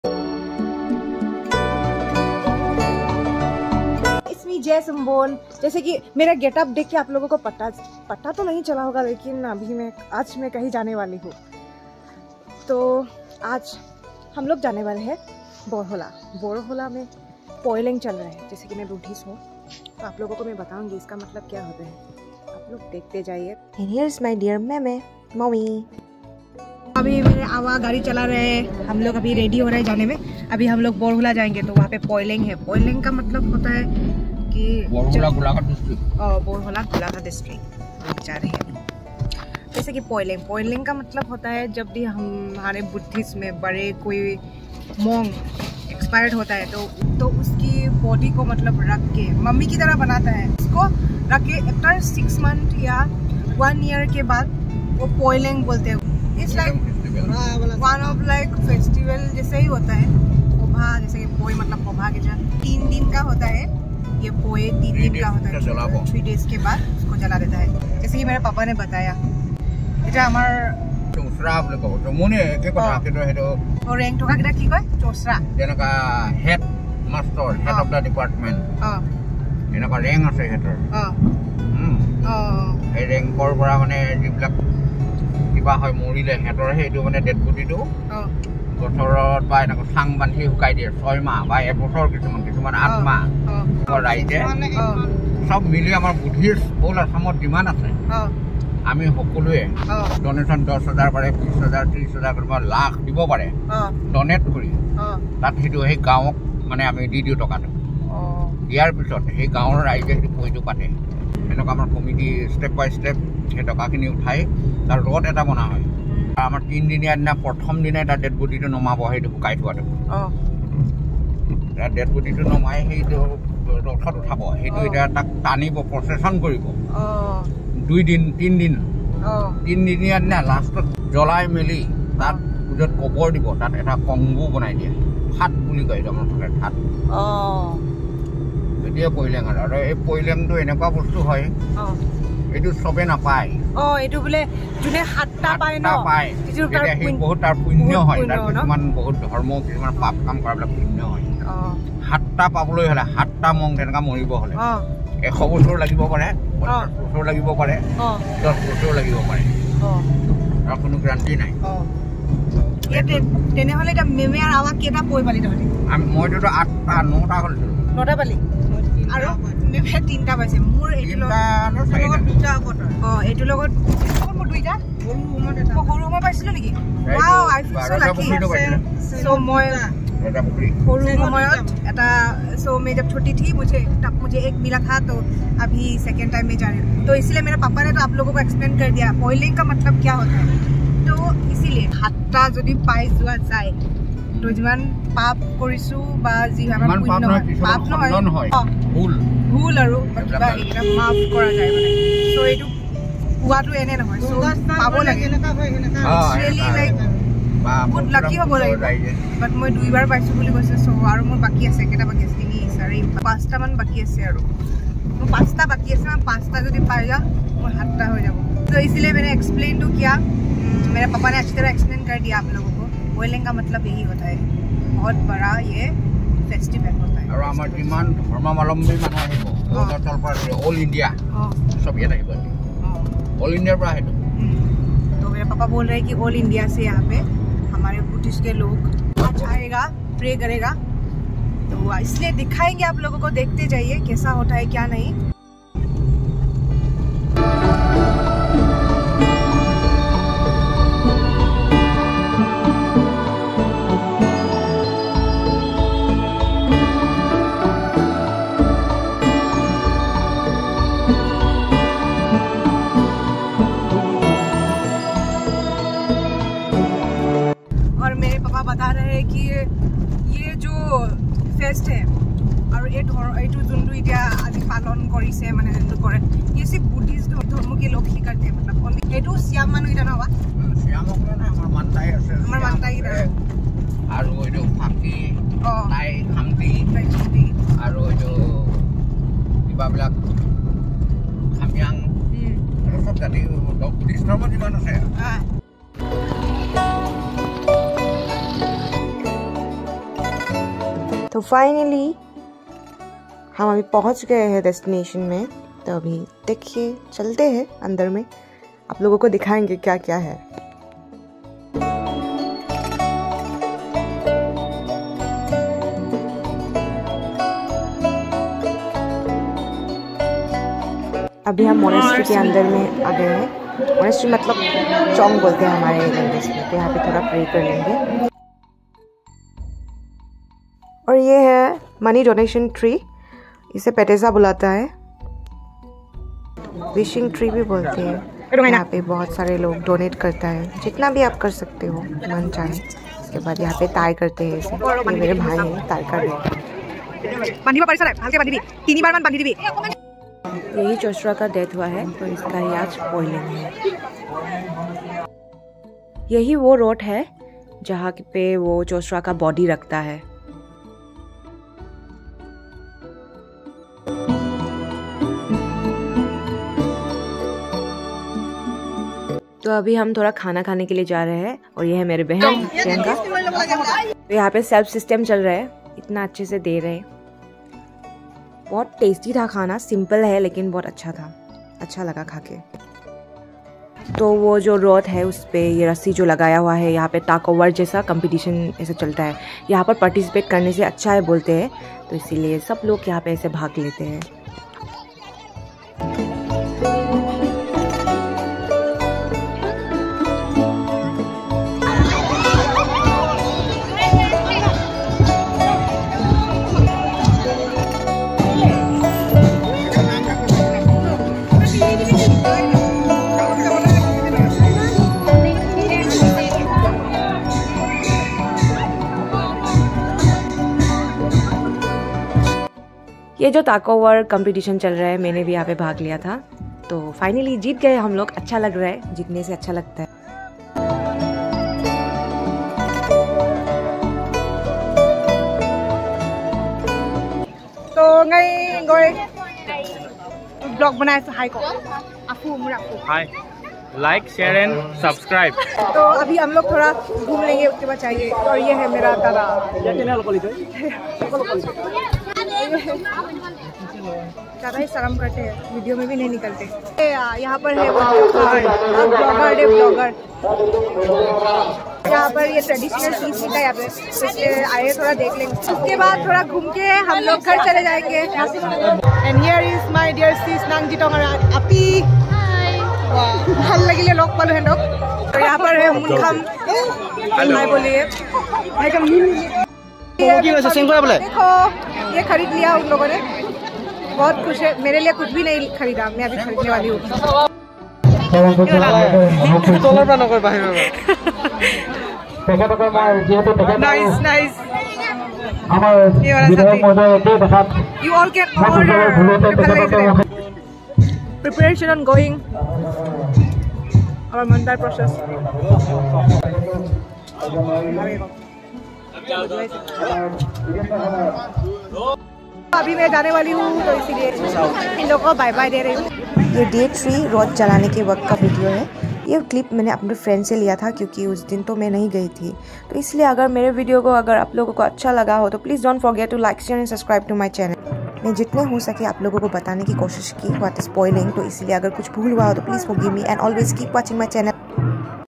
🙏🏻@Jaysumbon.buddhistculture miyafongpoi borholashyamgaon golaghatdist.
Hello evryone I am here again with my another new vlog of our Buddhist culture PoiLeng Borhola do watch out my vlog till the end to know about PoiLeng 😊🙏🏻💕.@Jaysumbon.